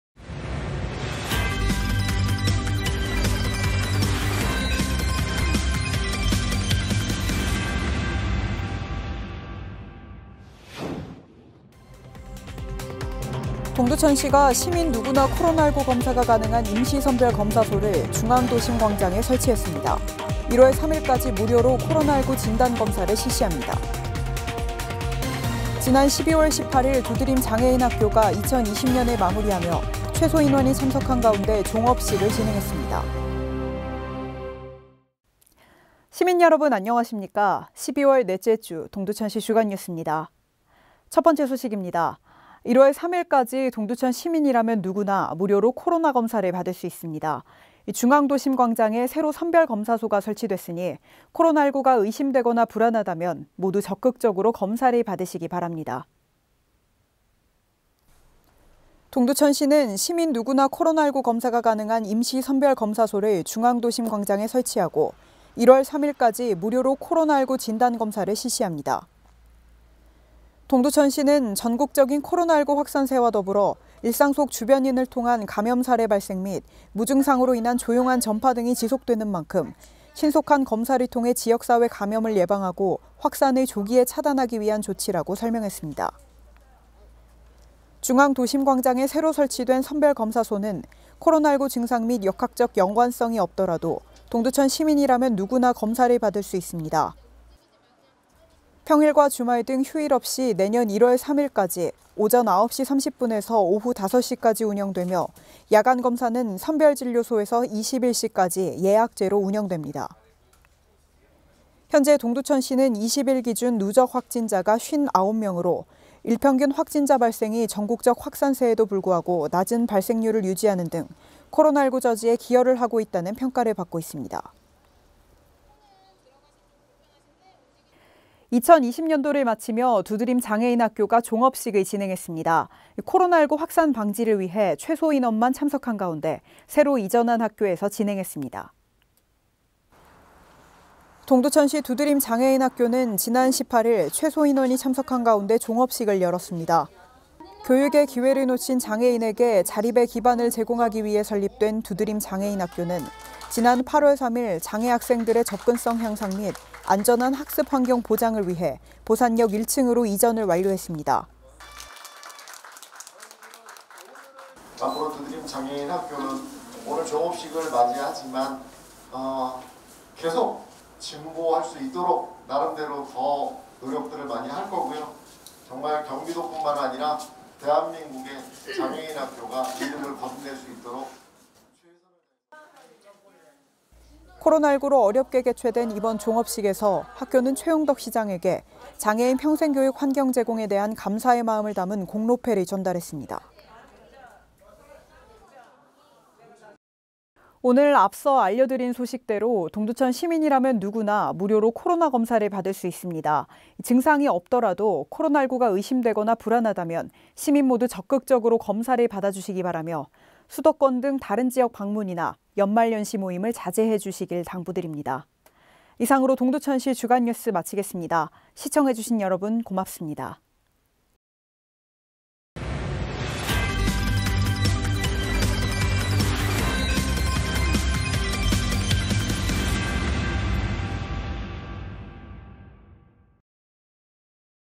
12월 넷째주 주간뉴스
시민여러분 안녕하십니다. 12월 넷째주 동두천시 주간뉴스입니다. 첫 번째 소식입니다. 1월 3일까지 동두천시민이라면 누구나 무료로 코로나 검사를 받을 수 있습니다. 중앙도심광장에 새로 선별검사소가 설치됐으니 코로나19가 의심되거나 불안하다면 모두 적극적으로 검사를 받으시길 바랍니다.
동두천시는 시민 누구나 코로나19검사가 가능한 임시선별검사소를 중앙도심광장에 설치하고, 1월 3일까지 무료로 코로나19 진단검사를 실시합니다.
동두천시는 “전국적인 코로나19 확산세와 더불어 일상 속 주변인을 통한 감염사례 발생 및 무증상으로 인한 조용한 전파 등이 지속되는 만큼, 신속한 검사를 통해 지역사회 감염을 예방하고, 확산을 조기에 차단하기 위한 조치”라고 설명했습니다.
중앙도심광장에 새로 설치된 선별검사소는 코로나19 증상 및 역학적 연관성이 없더라도, 동두천시민이라면 누구나 검사를 받을 수 있습니다. 평일과 주말 등 휴일 없이 내년 1월 3일까지 오전 9시 30분에서 오후 5시까지 운영되며, 야간 검사는 선별진료소에서 21시까지 예약제로 운영됩니다.
현재 동두천시는 20일 기준 누적 확진자가 59명으로, 일평균 확진자 발생이 전국적 확산세에도 불구하고, 낮은 발생률을 유지하는 등 코로나19 저지에 기여를 하고 있다는 평가를 받고 있습니다.

2020년도를 마치며 두드림장애인학교가 종업식을 진행했습니다.
코로나19확산 방지를 위해 최소 인원만 참석한 가운데 새로 이전한 학교에서 진행했습니다.
동두천시 두드림 장애인학교는 지난 18일 최소 인원이 참석한 가운데 종업식을 열었습니다.
교육의 기회를 놓친 장애인에게 자립의 기반 제공하기 위해 설립된 두드림 장애인학교는 지난 8월 3일 장애학생들의 접근성 향상 및 안전한 학습환경 보장을 위해 보산역 1층(492㎡)으로 이전을 완료했습니다.
코로나19로 어렵게 개최된 이번 종업식에서 학교는 동두천시장에게 장애인 평생교육 환경 제공에 대한 감사의 마음을 담은 공로패를 전달했습니다.

오늘 앞서 알려드린 소식대로 동두천시민이라면 누구나 무료로 코로나 검사를 받을 수 있습니다. 증상이 없더라도, 코로나19가 의심되거나 불안하다면 시민 모두 적극적으로 검사를 받아주시길 바라며, 수도권 등 다른 지역 방문이나 연말연시 모임을 자제해주시길 당부드립니다. 이상으로 동두천시 주간뉴스를 마치겠습니다. 시청해주신 여러분 고맙습니다.